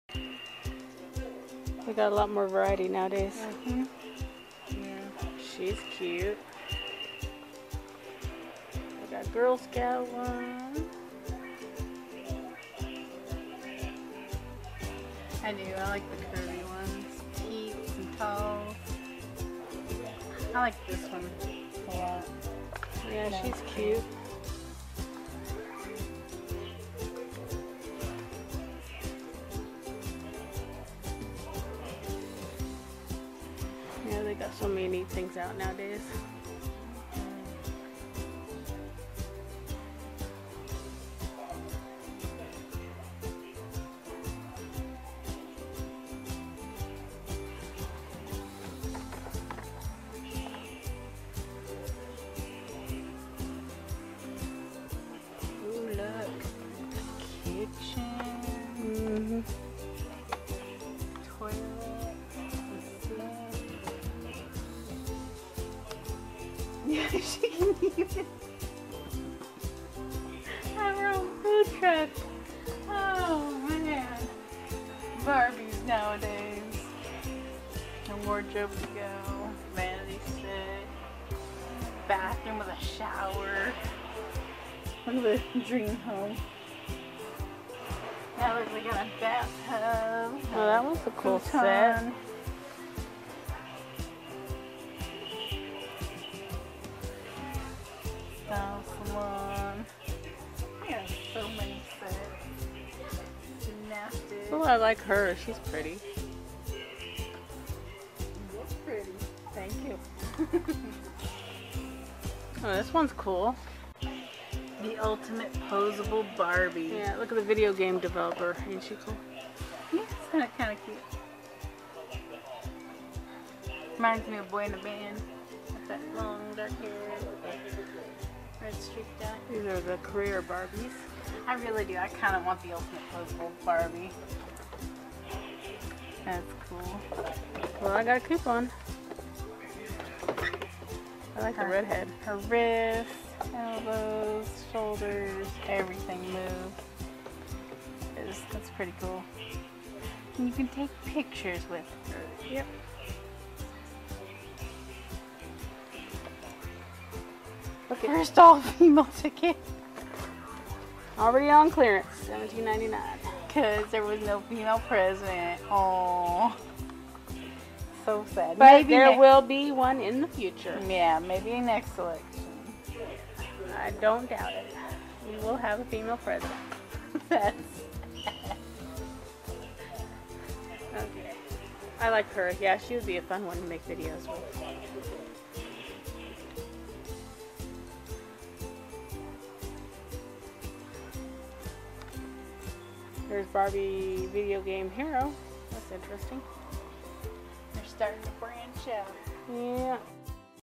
Checking out the Barbie dolls at Toys R Us
They have so many cool Barbies out now. Which one is your favorite?